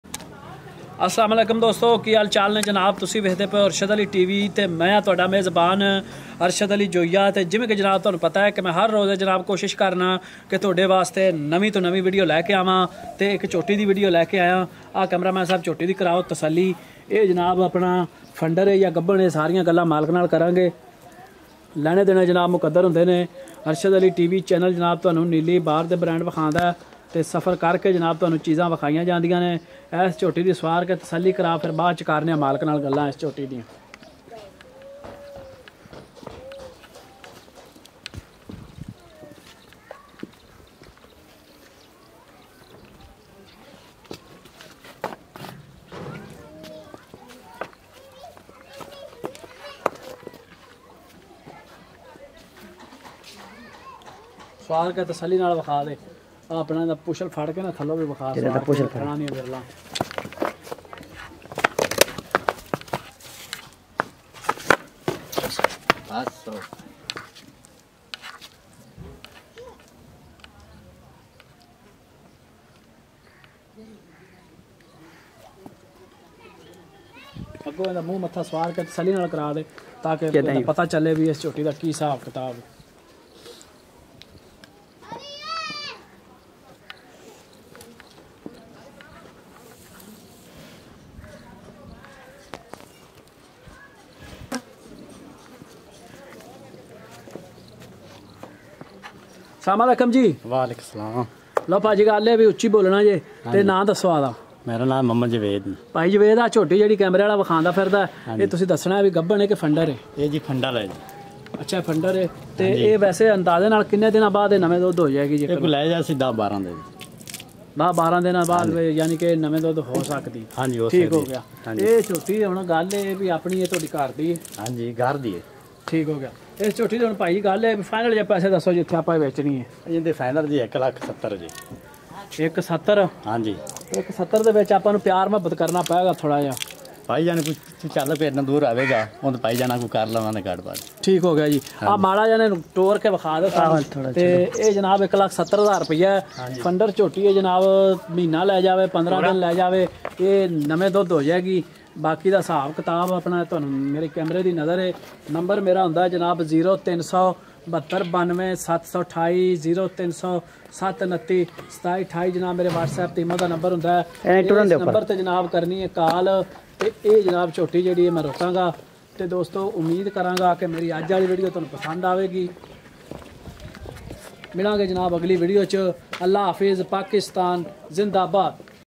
Watch nili ravi buffalo for sale 30-11-2020 in punjab Pakistan on YouTube
Nili ravi buffalo for sale in punjab Pakistan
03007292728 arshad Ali